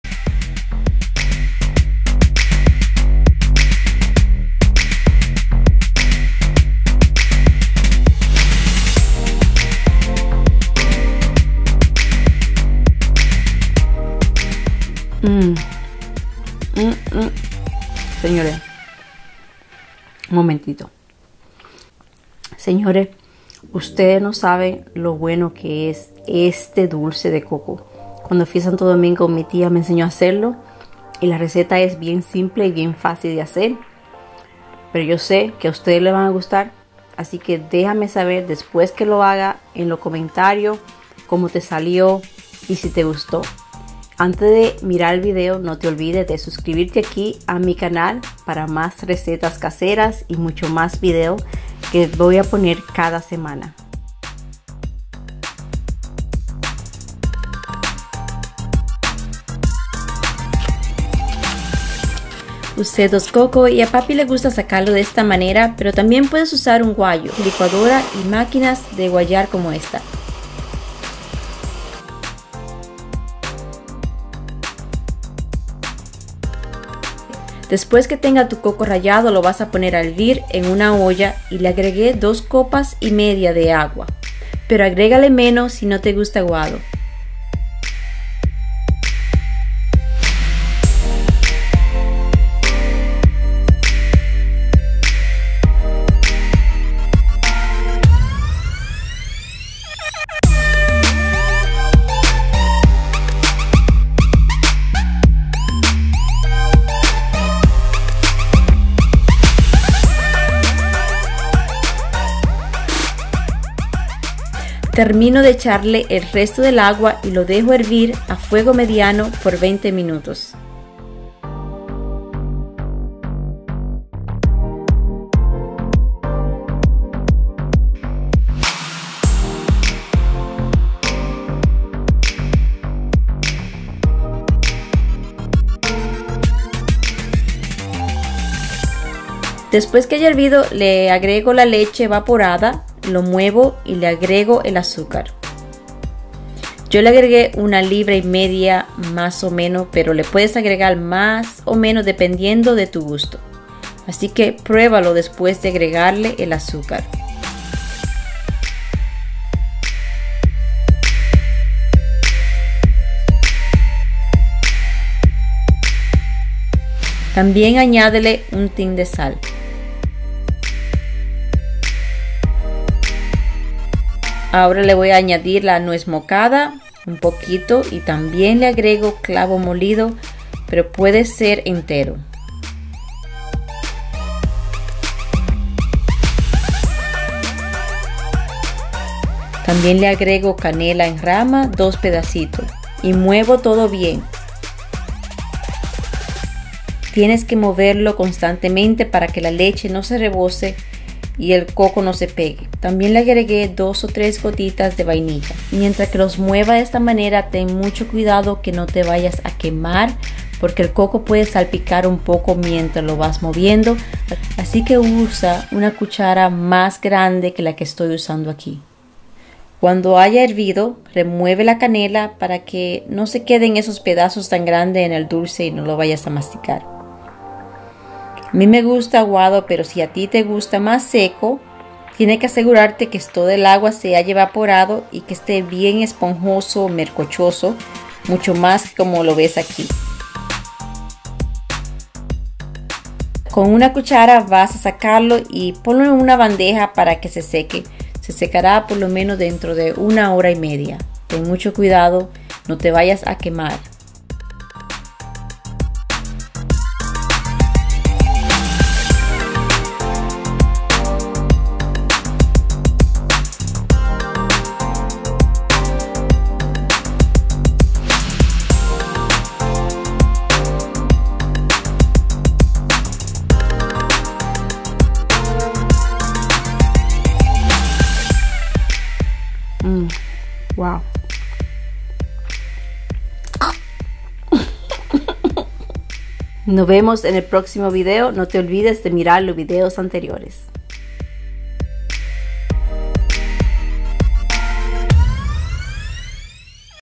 Como hacer dulce de Coco / Turron de coco / Dulce Dominicano
Dulce de coco: Manjar de coco: PEQUEÑOS BOCADILLOS MANJARES TROPICALES.. Aqui Veras Como hacer el dulce de coco dominicano paso a paso. Cómo hacer turrón de coco.

Esta es una receta que me enseñó mi tía para hacer un dulce rico de coco. Yo no soy muy dulcera pero este turrón es mi favorito.  Este turrón lo puedes hacer en la navidad para compartir con la familia y en cualquier ocasión.

Apoyame con tu suscripción Birmabibi
Snapchate Birmbibi
Twitter Birmabb
Camera Sony a 5100
Edited with Sony Movie Studios
Edited by me Birmabibi